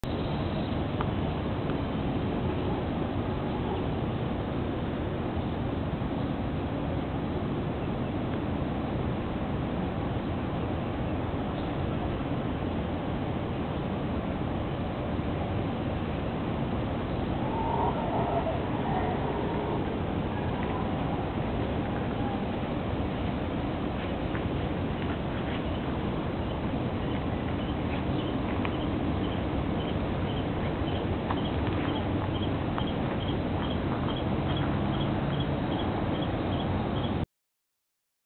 Panama City- Skyline
View of the Panama City Skyline from the rainforest in el Parque Metropolitano